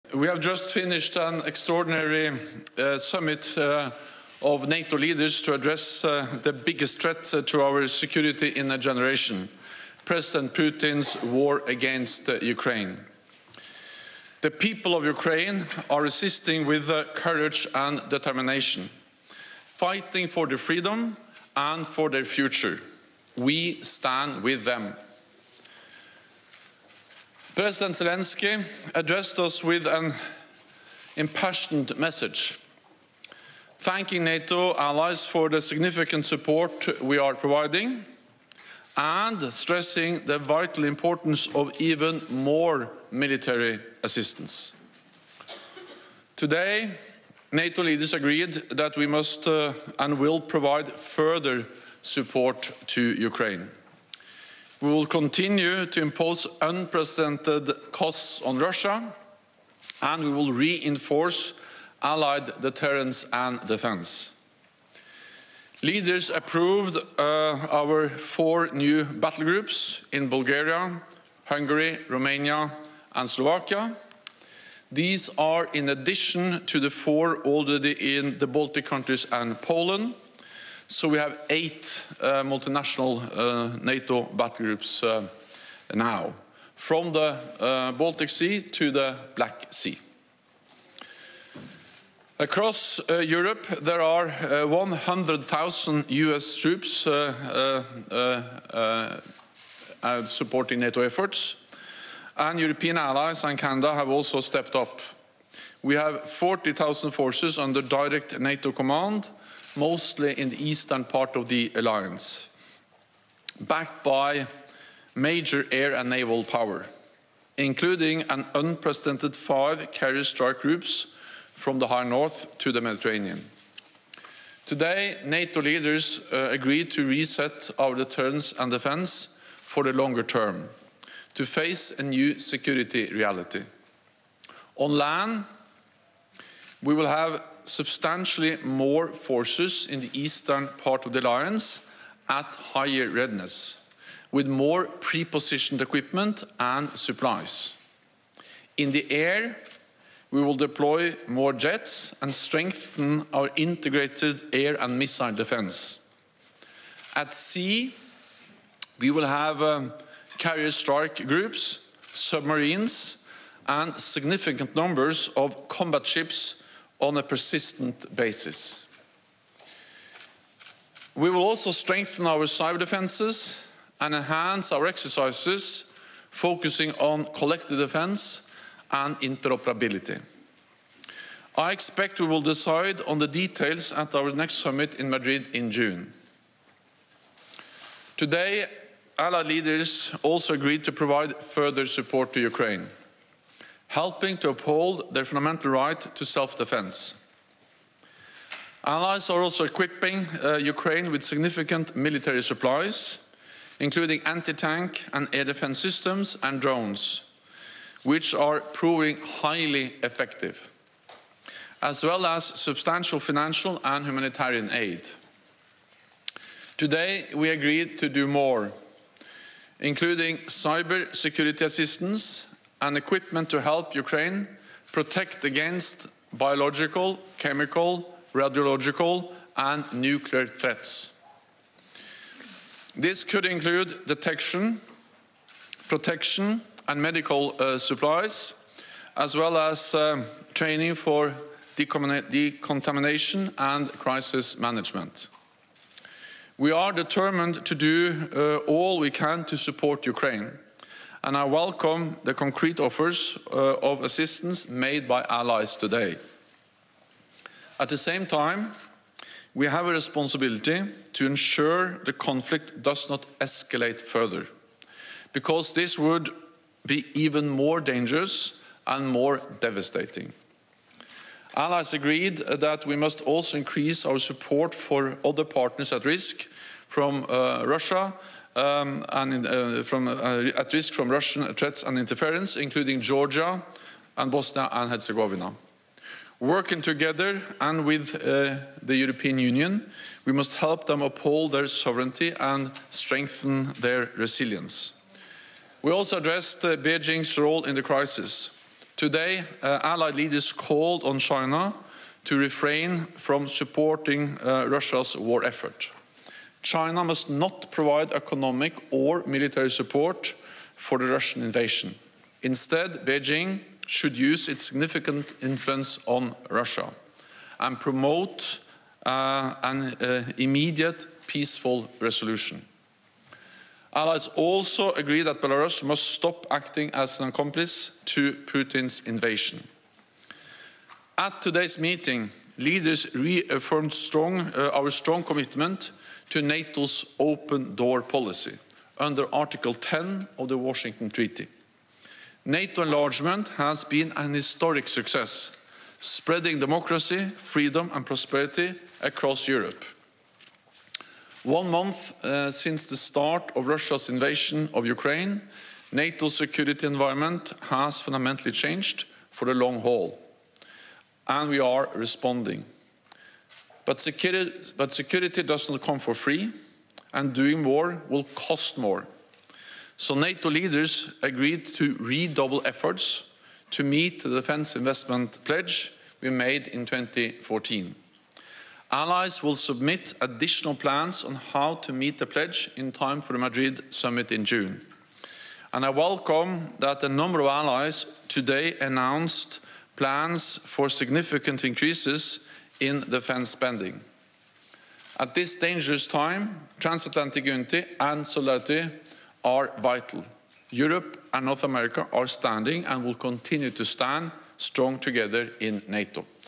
Ukraine War: NATO steps up support for Ukraine on land, sea and air
The General Secretary of NATO Jens Stoltenberg sets out the plan to give more support to Ukraine.

As well as strengthening battle groups from "the Baltic Sea to the Black Sea", NATO will bolster efforts on land, in the air, at sea and on the cyber front line with more ground troops, anti-tank systems, drones and jets to the areas around Ukraine.

He also said they would give "equipment to help Ukraine protect against biological, chemical, radiological and nuclear threats".

Mr Stoltenberg chastised Belarus for acting as an 'accomplice' to Russia, and said Beijing must do more to use it's influence over Russia "to promote peace".